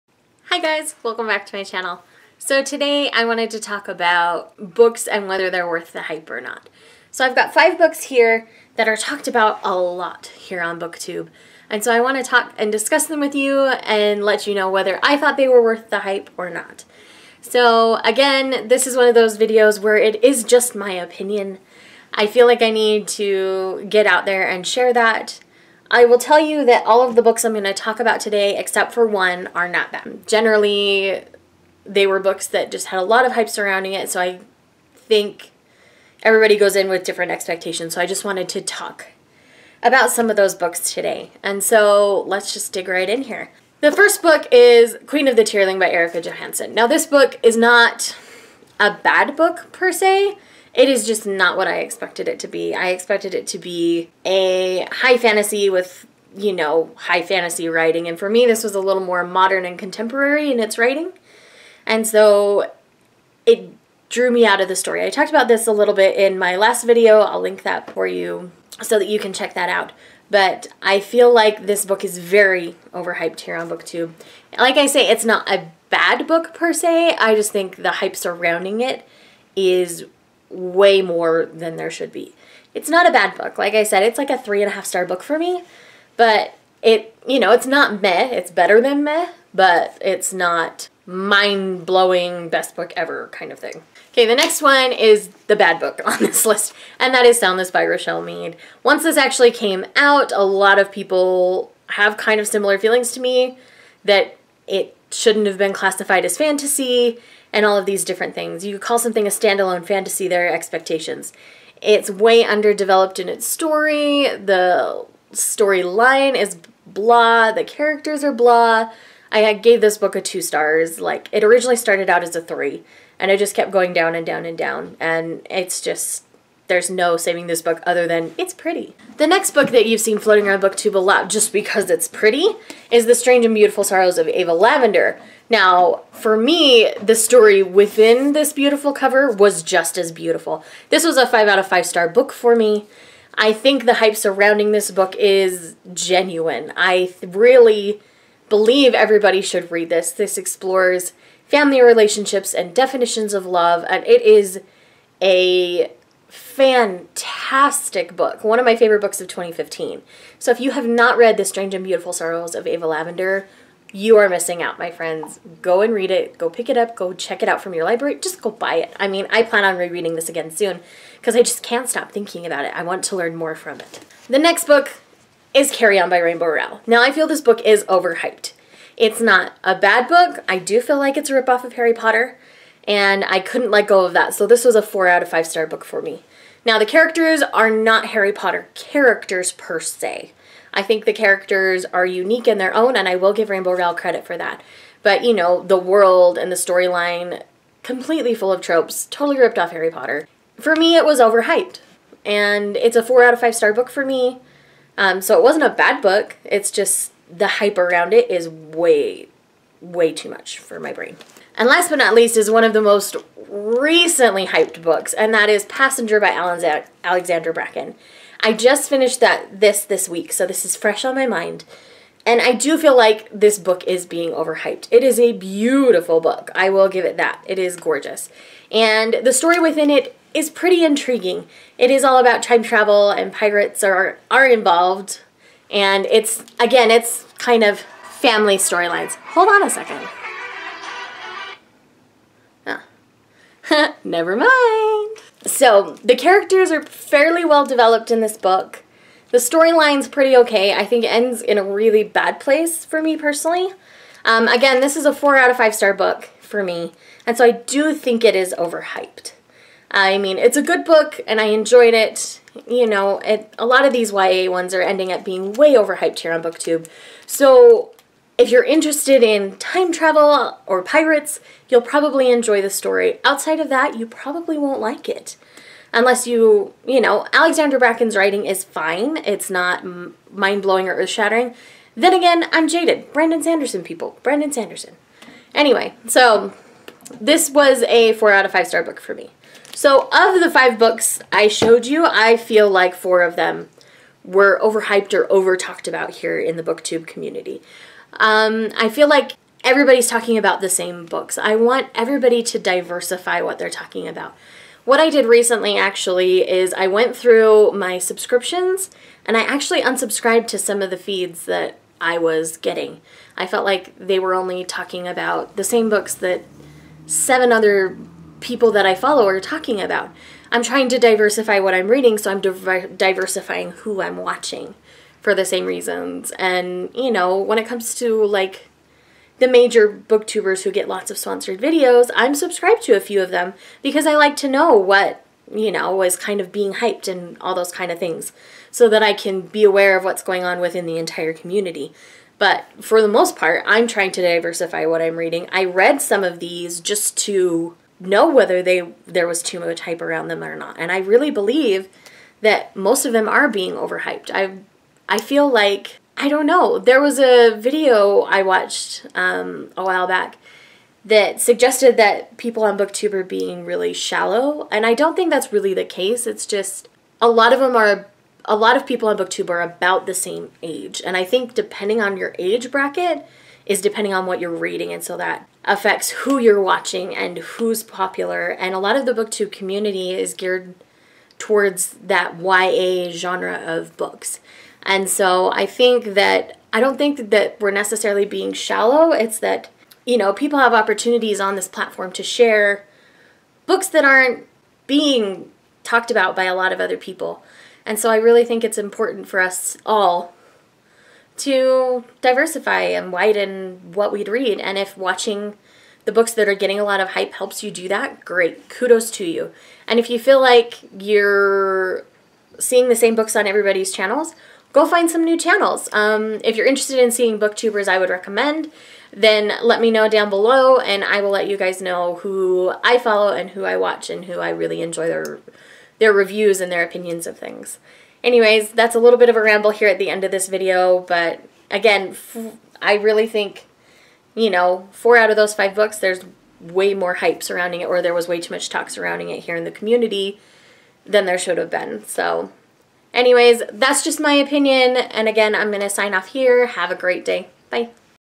Worth the Hype?? + A Booktube Quandry
Here are my opinion on whether 5 books I've read recently were worth the hype.  It lead me to other thoughts, which I share towards the end.  Let me know your thoughts!